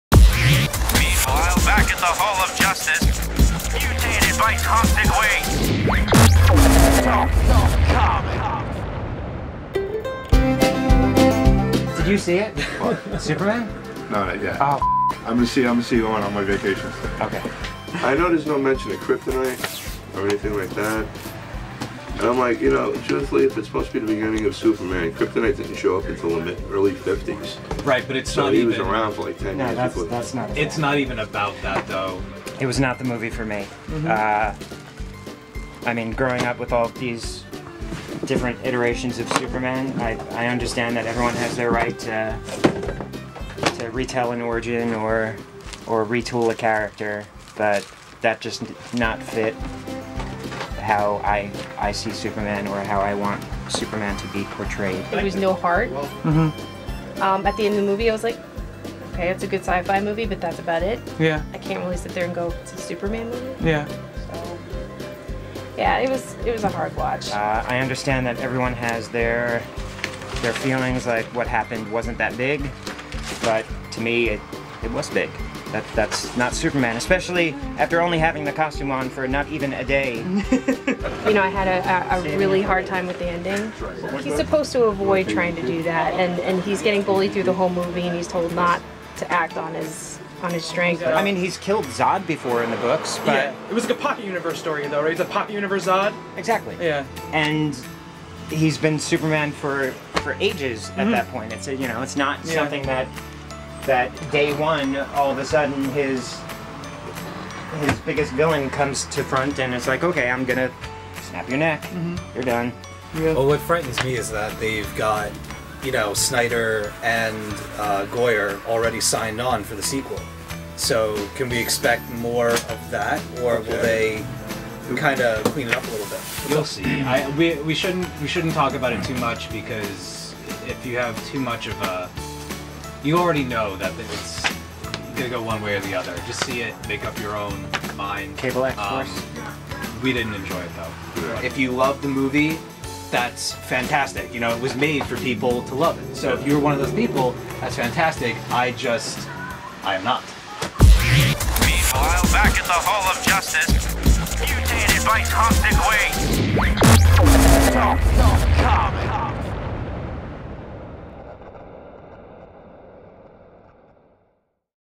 Shop Talk: Man of Steel
Talking Comics is at Tor Comics to talk Man of Steel the new Superman movie from Warner Bros. Beware Spoilers!